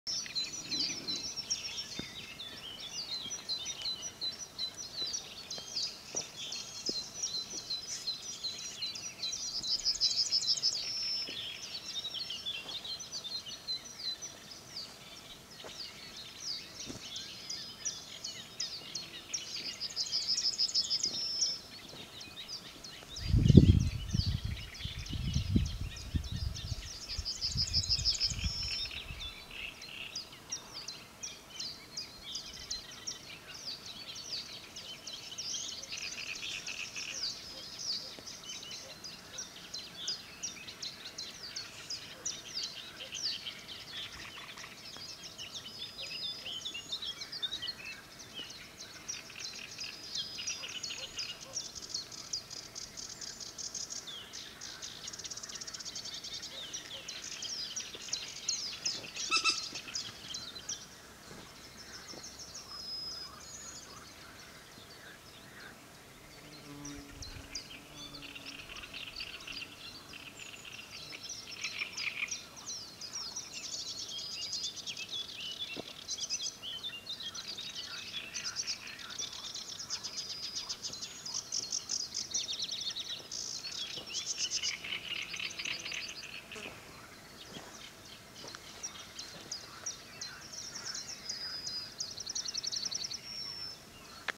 Zaskroniec zwyczajny
Zaskroniec zwyczajny – gatunek niejadowitego węża z rodziny połozowatych. Samica jest większa od samca i osiąga średnio długość do 1,5 metra, a samiec do 1 metra. Odnotowywano osobniki dłuższe. Żywią się płazami, rybami albo małymi gryzoniami, które połykają bez uprzedniego uśmiercania. Zaskrońce występują w całej Europie.